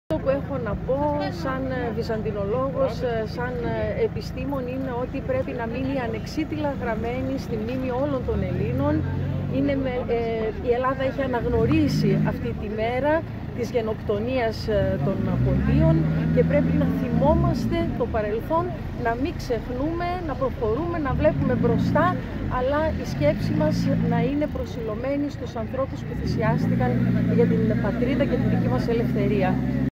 Μαρία Τζιάτζη, Πρόεδρος Τμήματος Ελληνικής Φιλολογίας ΔΠΘ
Δηλώσεις της Προέδρου του Τμήματος Ελληνικής Φιλολογίας Μαρίας Τζιάτζη μετά την κατάθεση στεφάνων τιμώντας την ημέρα μνήμης της Ποντιακής Γενοκτονίας